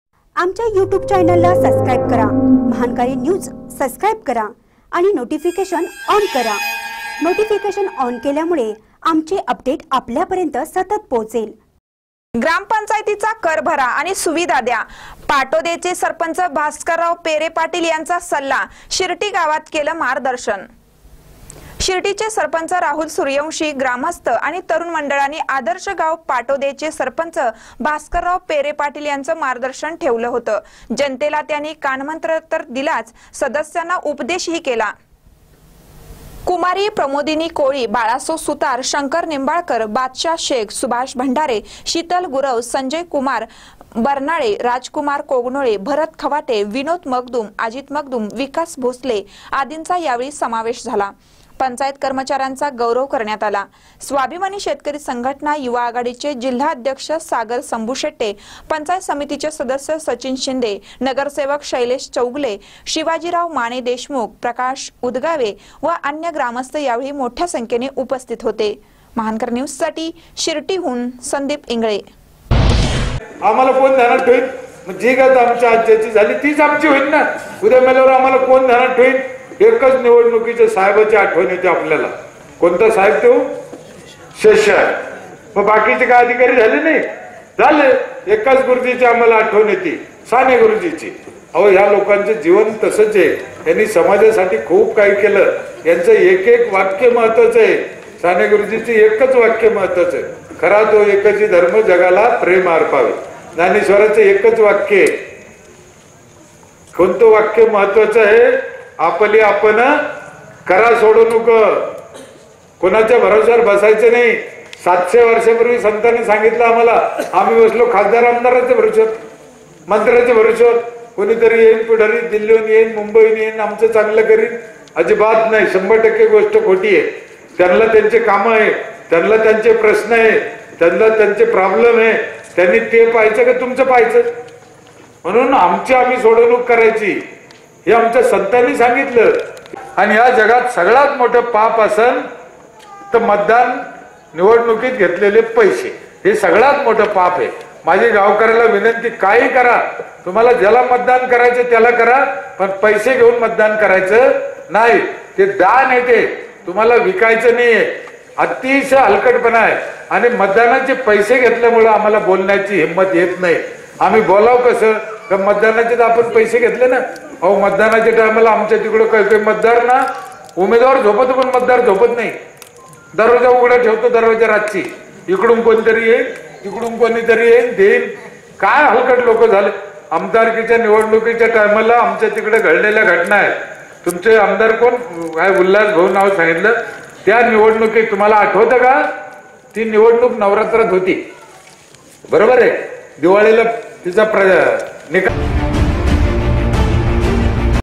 ग्रामपंचायतीचा कर भरा आणि सुविधा घ्या. पाटोदाचे सरपंच भास्करराव पेरे पाटील यांचा सल्ला
ग्रामपंचायतीचा कर भरा आणि सुविधा घ्या. पाटोदाचे सरपंच भास्करराव पेरे पाटील यांचा सल्ला शिरटी गावाल केलं मार्गदर्शन